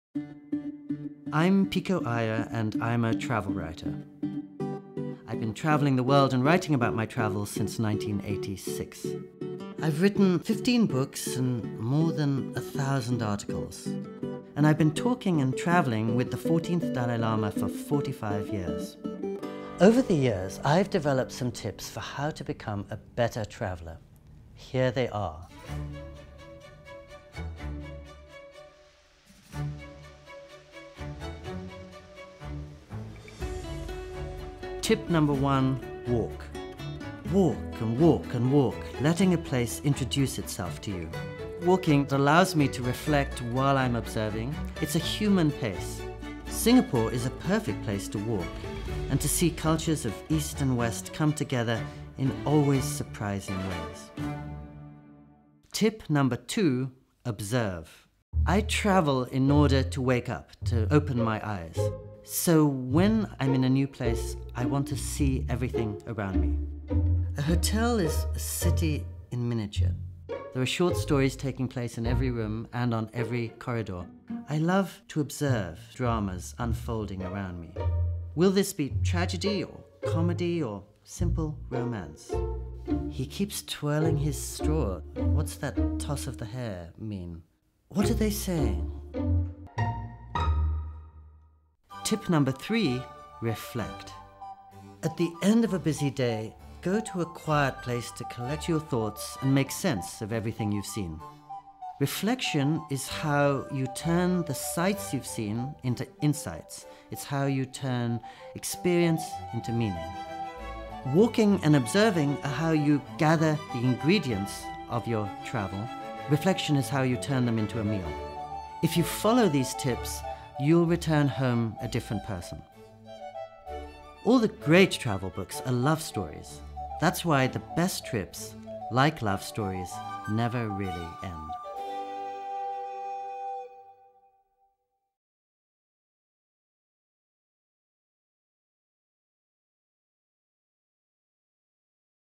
A Travel Writer Shares His Best Tips
Travel can be so rewarding. It’s the ultimate way to learn about different cultures and meet new people. How can you make sure you get the most out of your next trip? Pico Iyer, who has been writing about travel since 1986, has lots of great advice to share. Join the thoughtful globetrotter in Singapore to learn why the best way to get around many destinations is on foot, the benefits of being an observant traveler and the value of reflection.